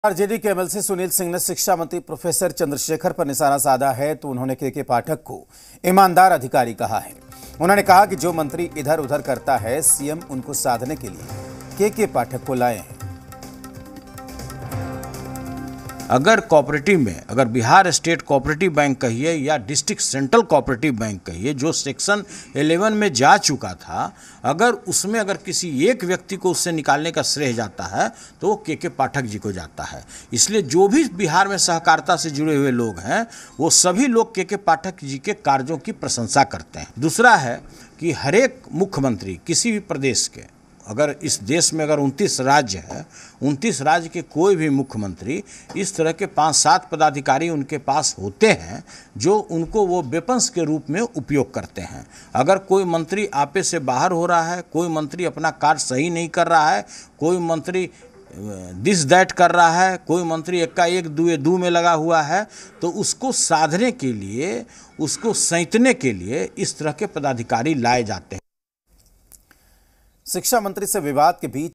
Chandrashekhar Vs KK Pathak: RJD MLC Sunil Singh का शिक्षा मंत्री Chandrashekhar पर वार | Bihar
RJD MLC Sunil Singh's attack on Education Minister Chandrashekhar...Sunil Singh told KK Pathak to be honest

Chandrashekhar Vs KK Pathak : RJD के MLC सुनील सिंह ने शिक्षा मंत्री प्रोफेसर चंद्रशेखर पर निशाना साधा है...वहीं उन्होंने केके पाठक को ईमानदार अधिकारी बताया है. उन्होंने कहा कि जो मंत्री इधर-उधर करता है...सीएम उनको साधने के लिए केके पाठक को लाए हैं...देखिए पूरी ख़बर...